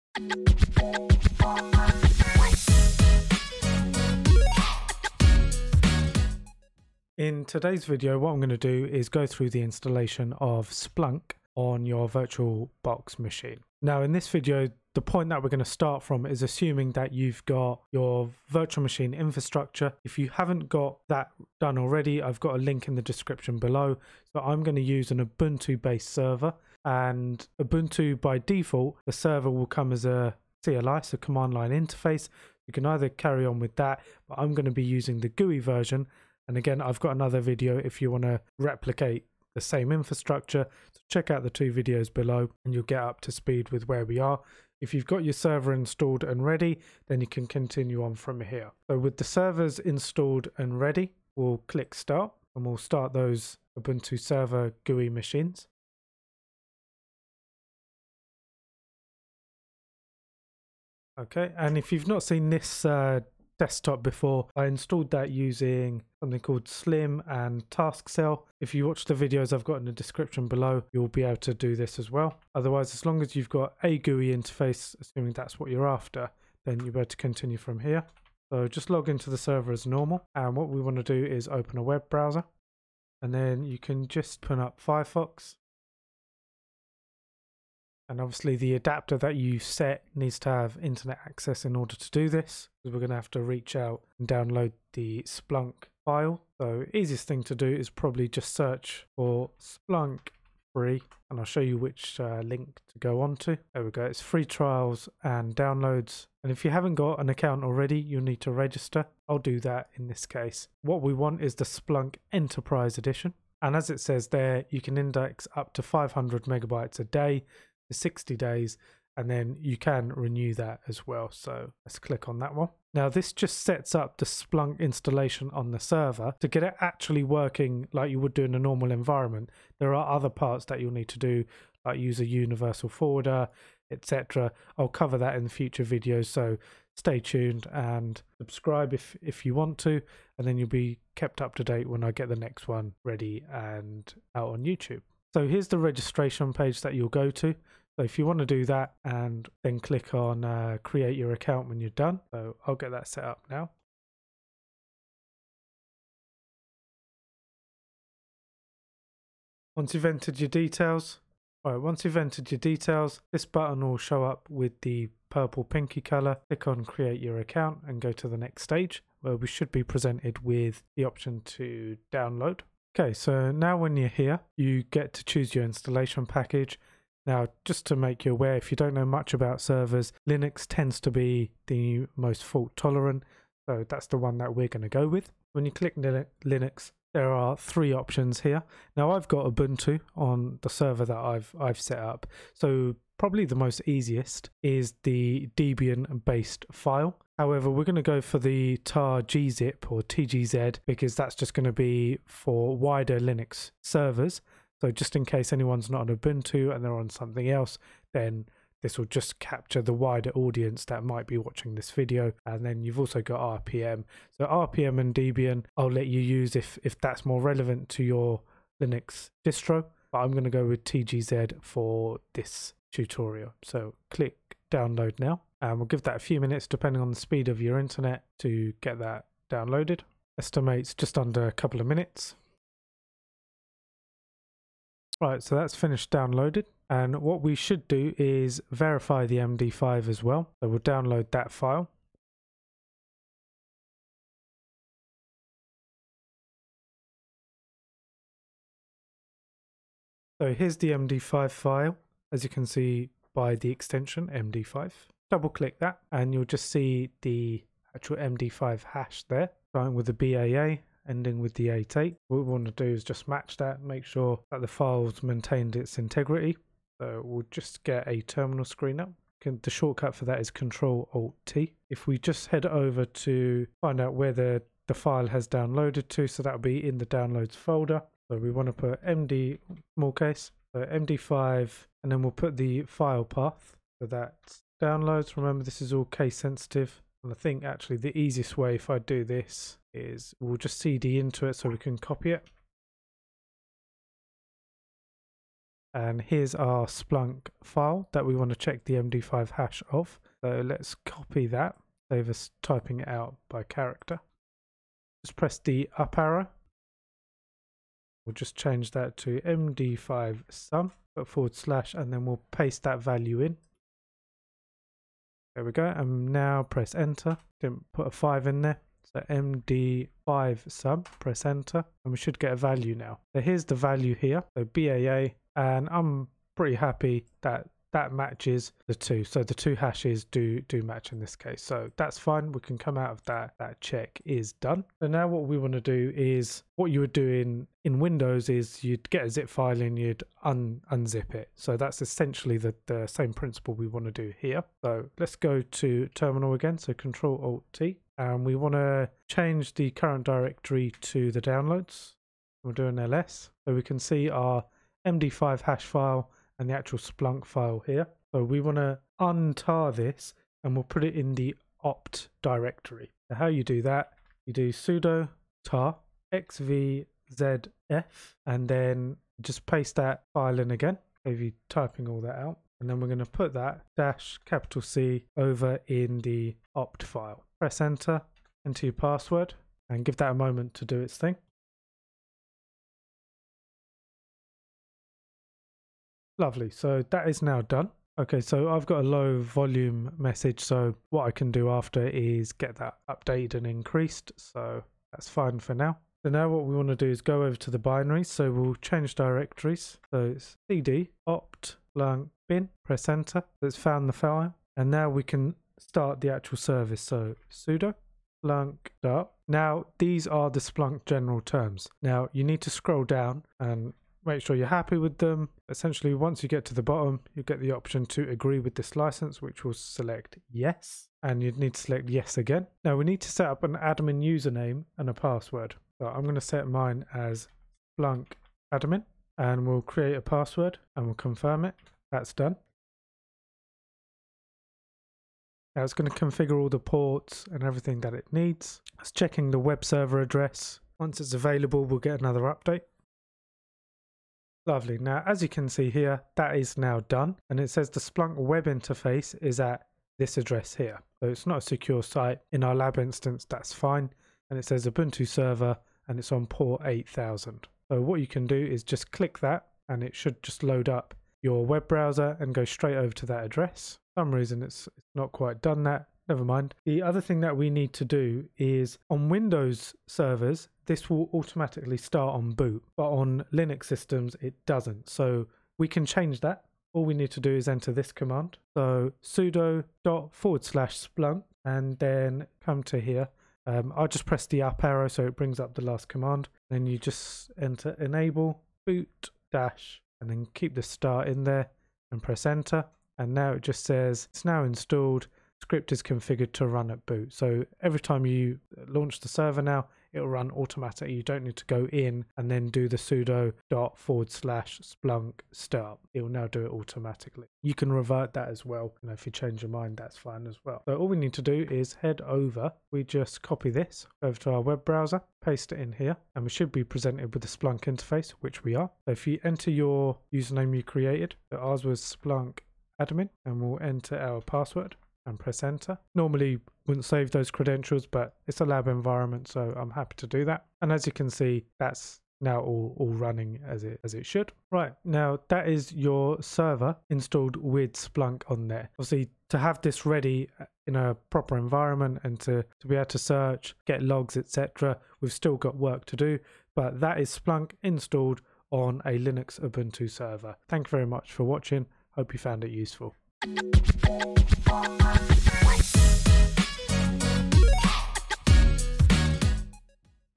How to install Splunk SIEM on an Ubuntu server
This video covers how to install Splunk Enterprise on an Ubuntu Server - which has a GUI using Slim.

Note: I found Slim to be unstable in some cases so re-installed my GUI as Ubuntu-Desktop version instead. This takes up more space than Slim but has been far more stable. If you want to setup GUI using Ubuntu-Desktop then watch this video;